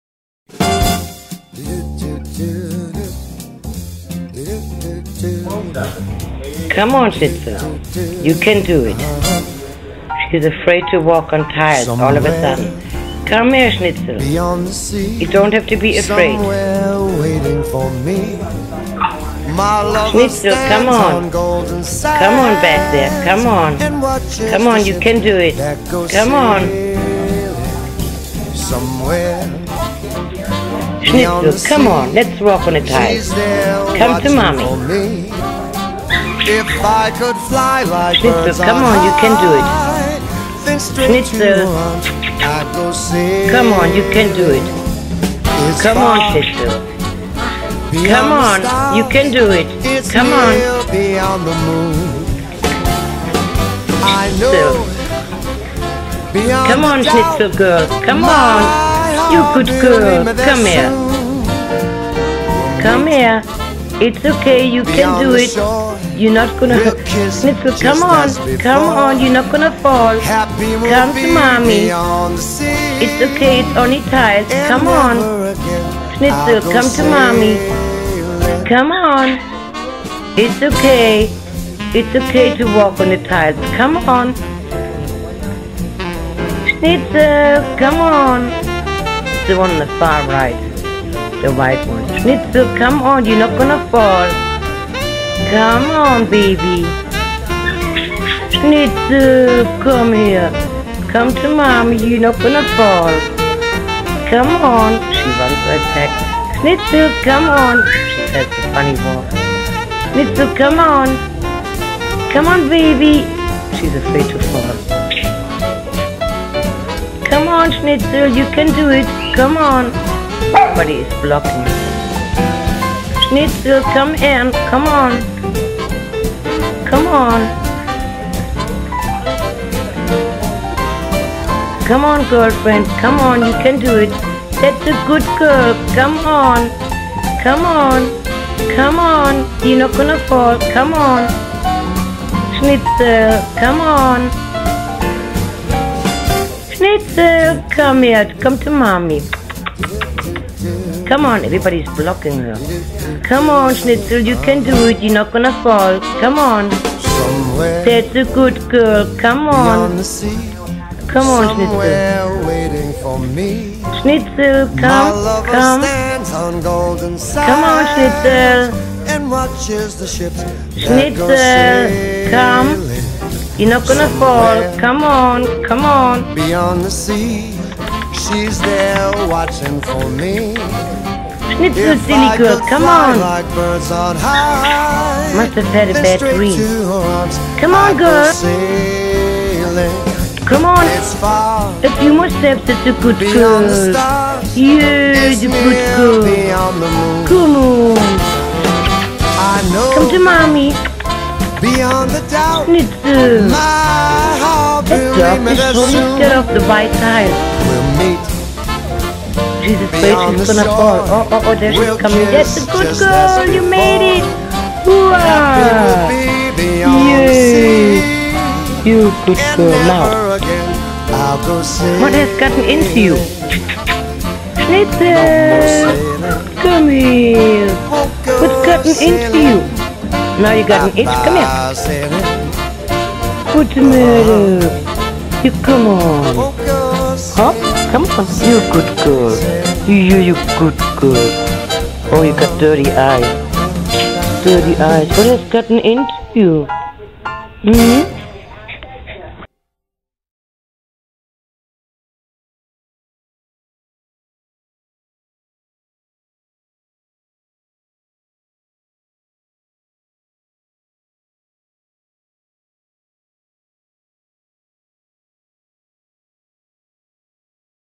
Schnauzer afraid to walk on Tile Floor
Schnitzel the Schnauzer, age 2.5 years woke up from nap time and then began to be afraid to walk on the tile floor. She must have had a bad dream.